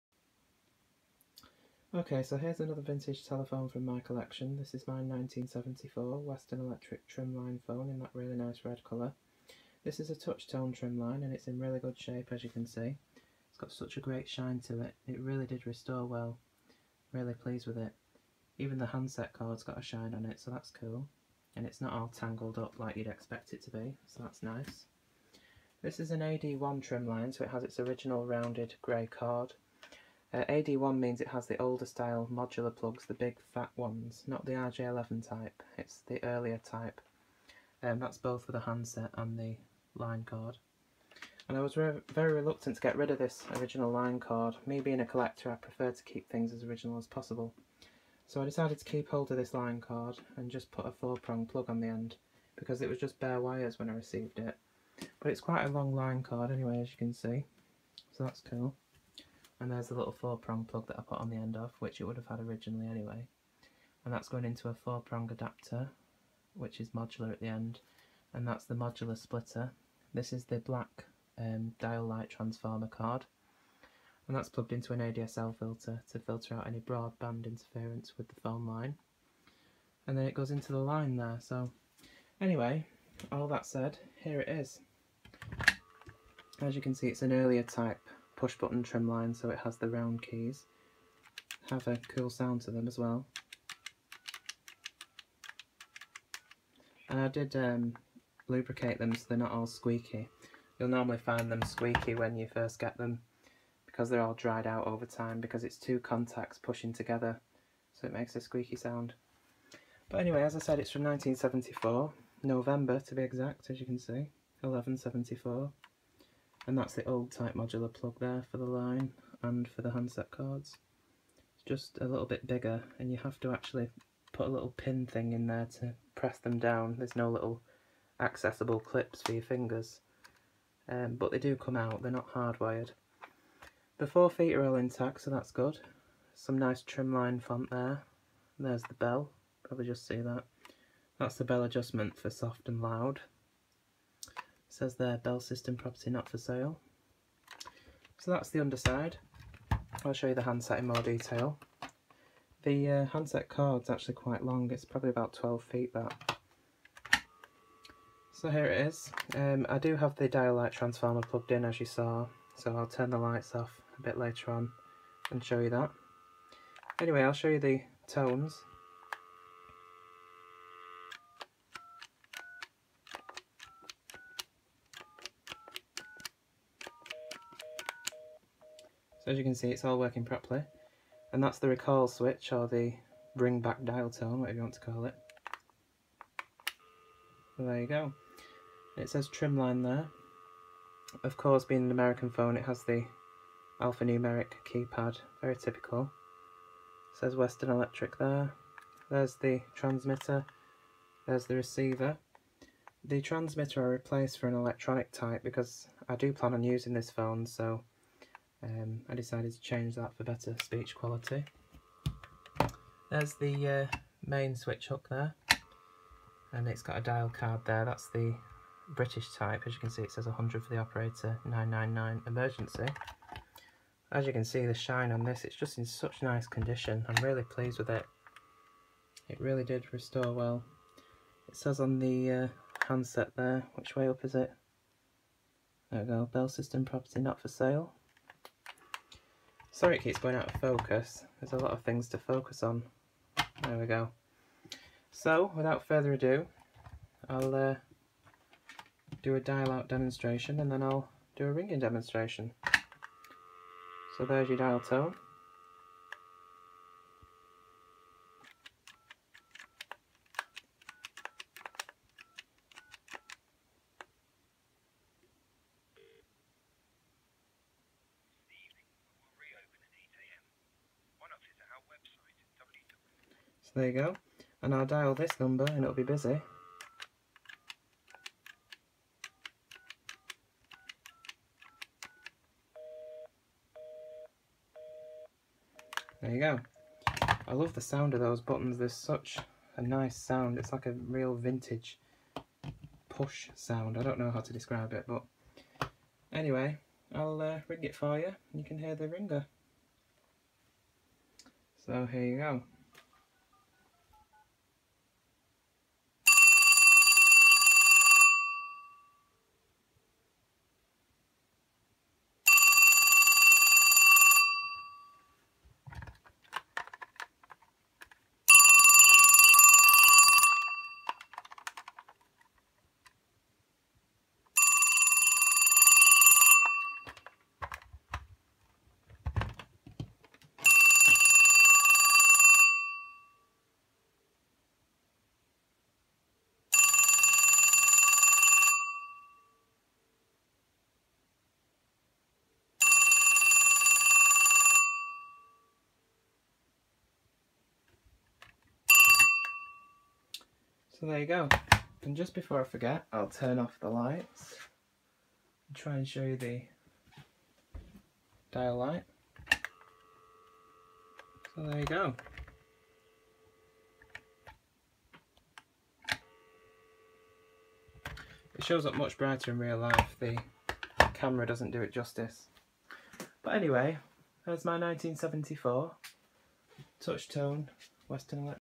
1974 Red Bell System Western Electric DTMF Desk Trimline Telephone
My camera died just a few seconds before I was about to end the video, but all I was going to say was "Western Electric Trimline"